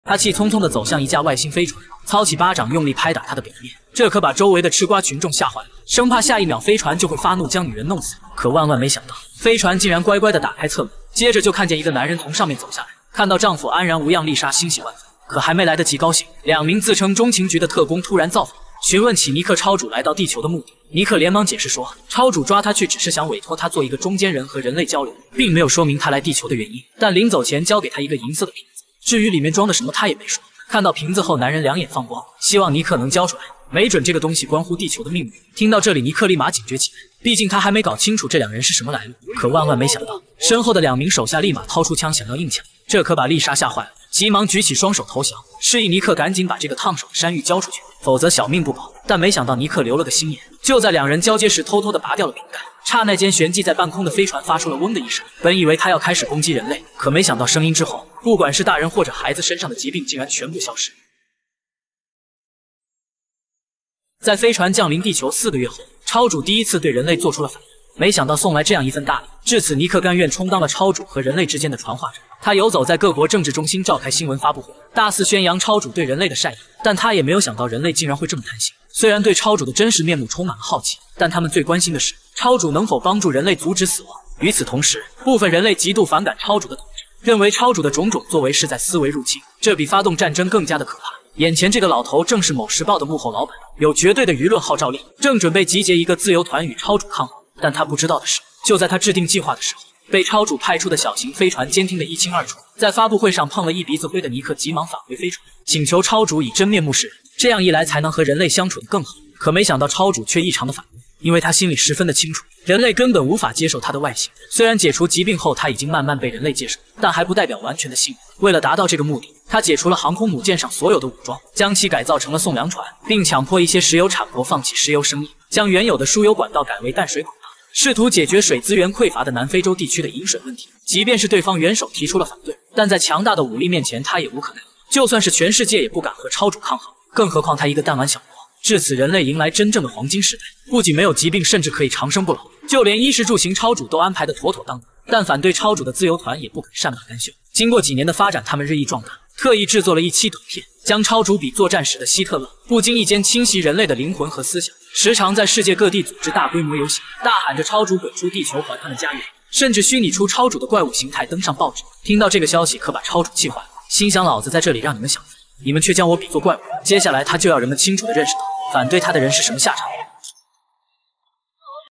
第3集：外星人侵入地球后，不殺一人，只抓了一個不起眼的農民！
愛情,喜劇,網絡大電影,自制電影,懸疑,科幻,推理,電影獨播,中國電影,電影解說,解說